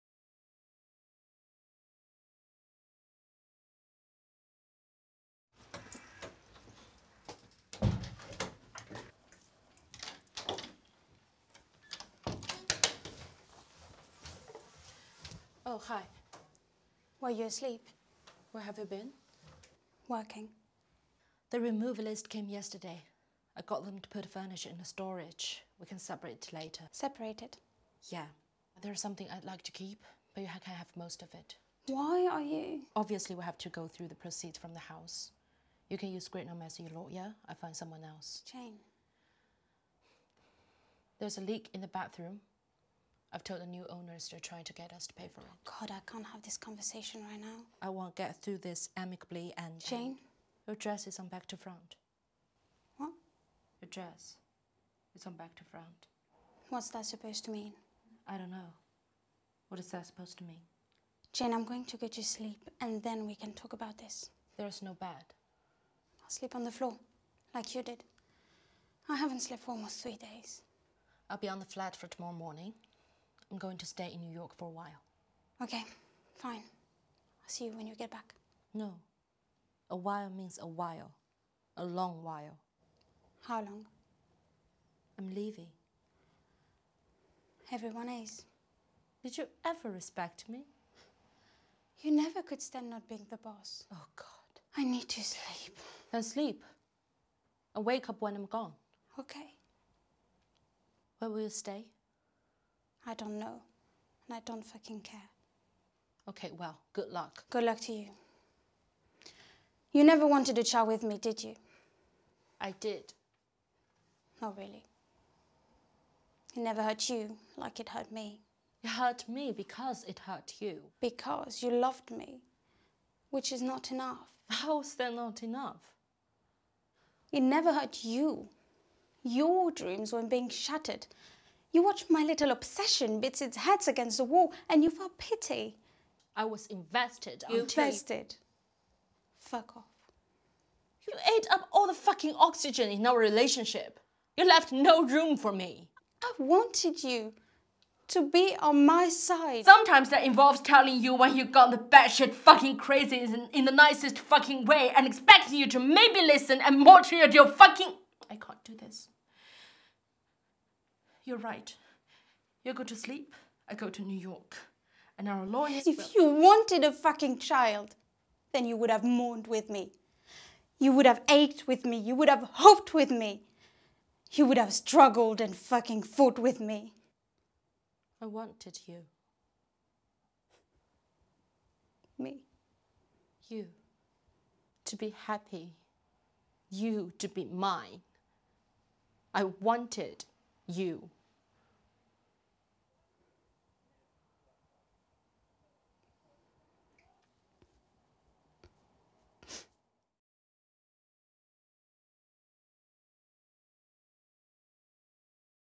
Drama Studio London -Yerma - Duologue Scene
A scene from Yerma directed by Gerard Johnson and with actor Wen Zhou.